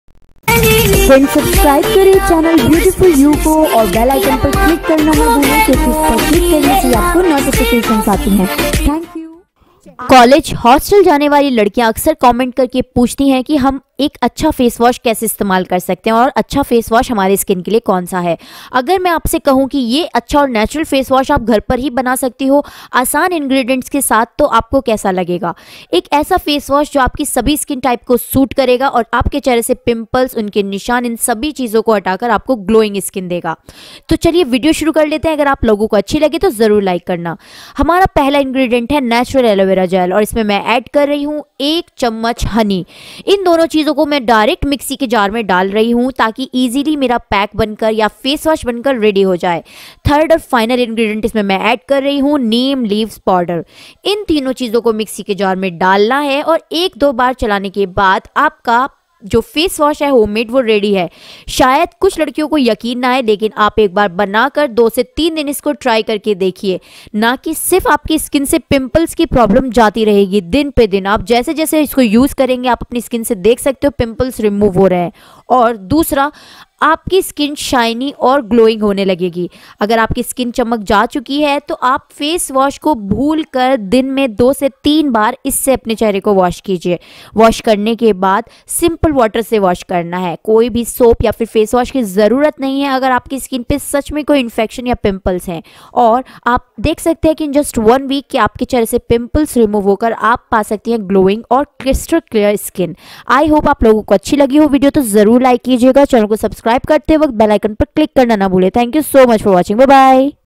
लड़कियां पिम्पल खत्म करने का Face wash घर पर बनाये,पिम्पल कभी नही होंगे,चेहरा ग्लो करेगा|Glowing skin
Neem helps in healing mild skin problems like rashes, irritation, burns and infections with its antibacterial and anti-inflammatory properties. It also helps in nourishing dry skin. Take two tablespoons of neem powder and sandalwood powder each, and add a little water and one tablespoon of rose water to make a paste.

“It contains antioxidants, enzymes, Vitamins A and C, and it is highly anti-inflammatory. It can help treat burns, acne and dry skin.” Chang added that when it comes to acne, Aloe Vera works best on superficial surface acne rather than cystic or deeper acne.

Wallace shares four of the main skin-saving benefits of honey.
Acne: Honey is naturally antibacterial, so it's great for acne treatment and prevention.
Aging: Full of antioxidants, it is great for slowing down aging.
Complexion boost: It is extremely moisturizing and soothing, so it helps create a glow.